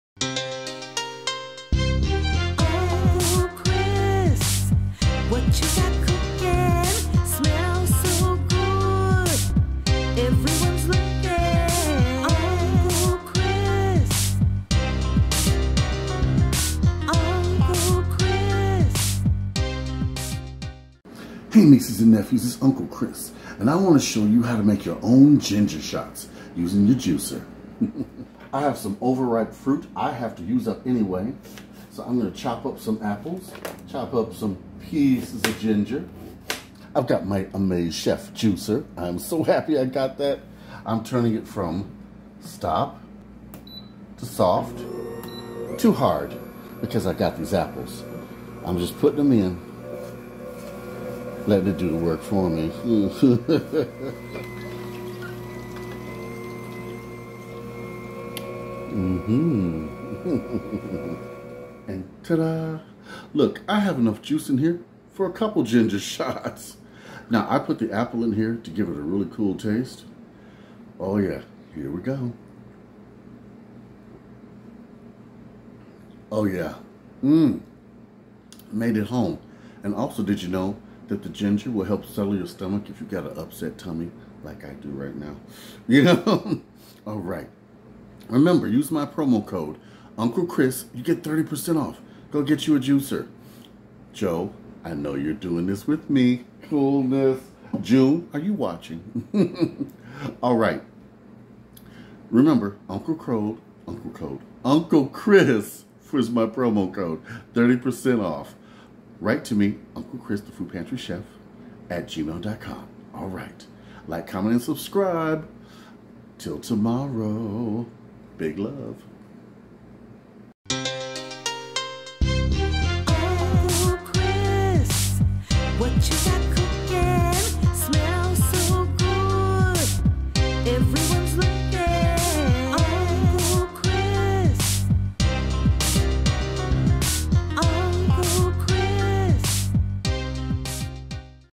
Food pantry ginger shots
Everyone has seen those ginger shots in the store.Now you can make them too with your juicer.I love the one from amazing chef.Get yours now and start making your own gender shots.Use my promo code to get thirty percent off

GET YOUR JUCIER HERE!!!